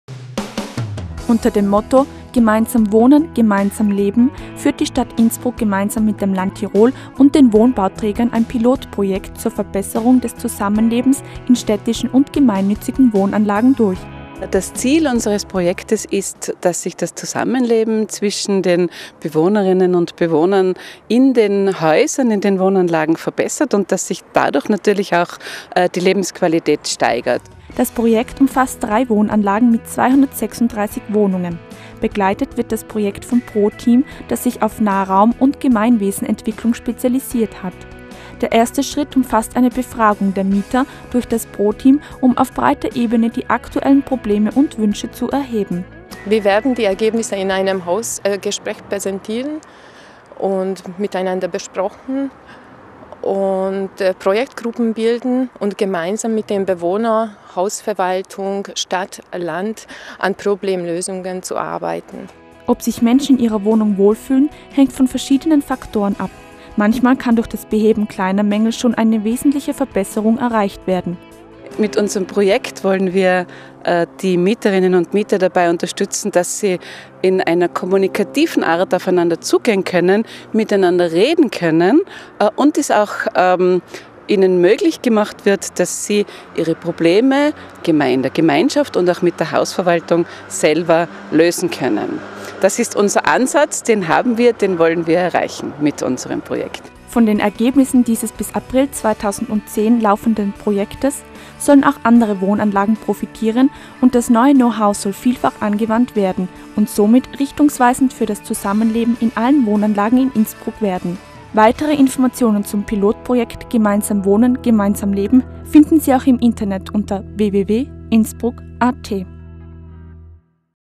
Innsbruck TV 09: Wohnprojekt
Gemeinsam Wohnen - gemeinsam Leben, Pilotprojekt zur Verbesserung des Zusammenlebens in städischen und gemeinnützungen Wohnanlagen